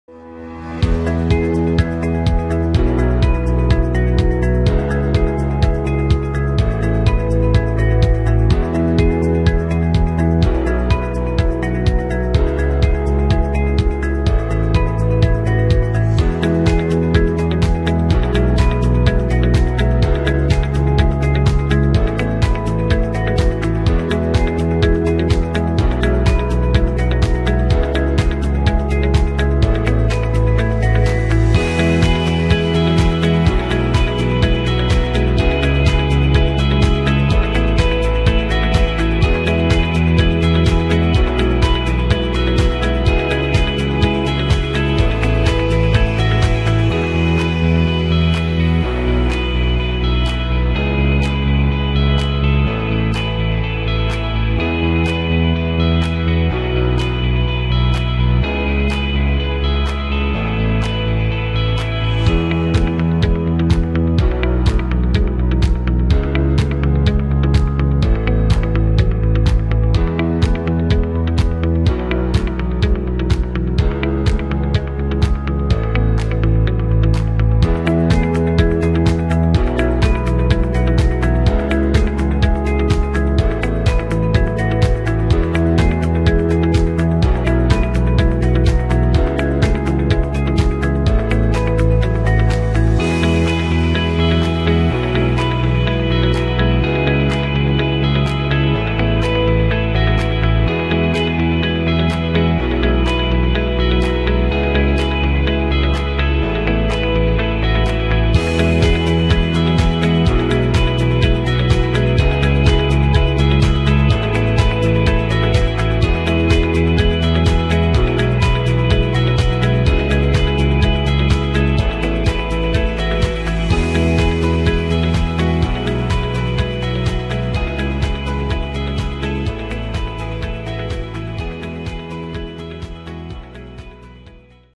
Tarzana Hills Home For Sale 4434 Nogales Dr
Elegant Home for Sale in Tarzana. 5bdrm 4bath, Pool, Spa and so much more.